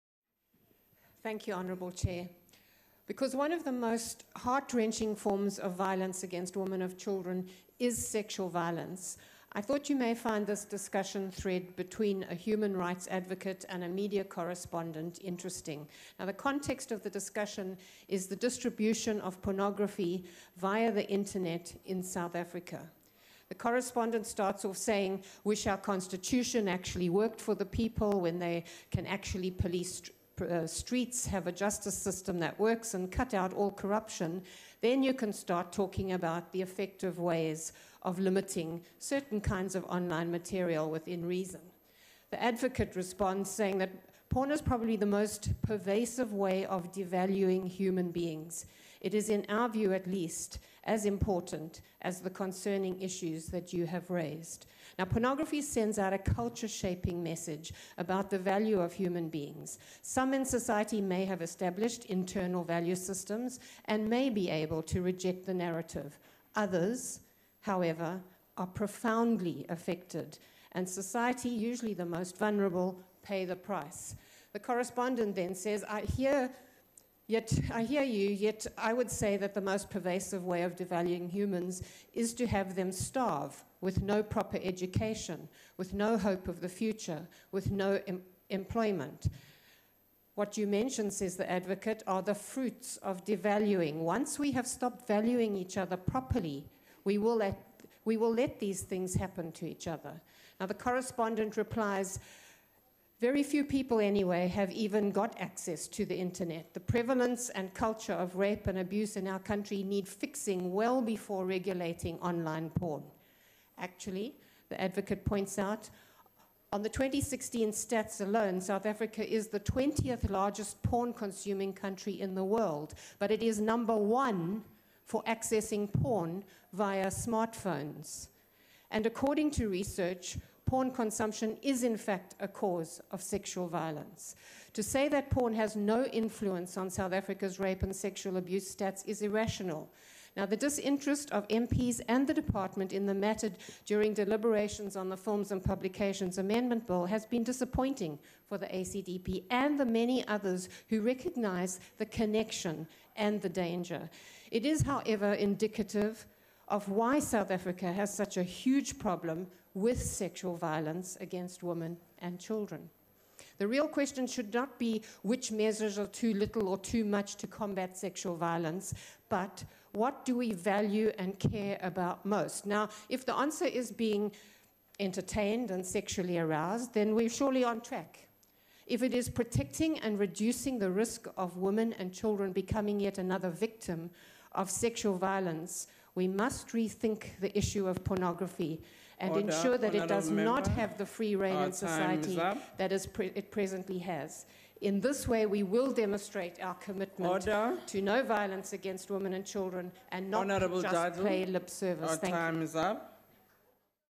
ACDP MP, Cheryllyn Dudley: Debate on 16 days of activism_211117
National Assembly debates
21 November 2016

Speaking on Tuesday’s debate on "16 Days of Activism for No Violence Against Women and Children: Count me in: Together moving a non-violent South African forward”, ACDP MP, Cheryllyn Dudley asserted that one of the most “heart-wrenching" forms of violence against women and children was sexual violence.

"Pornography is one of the most pervasive way of debasing human beings … it sends out a culture-shaping message about the value of human beings … once we have stopped valuing each other properly, we will let these things happen to each other …”

“South Africa is the 20th largest porn-consuming capital in the world, but it is number for accessing porn via smartphones. According to research, porn consumption is, in fact, a cause for sexual violence.”

“What do we value and care about most?"

Video courtesy of Parliament of the Republic of South Africa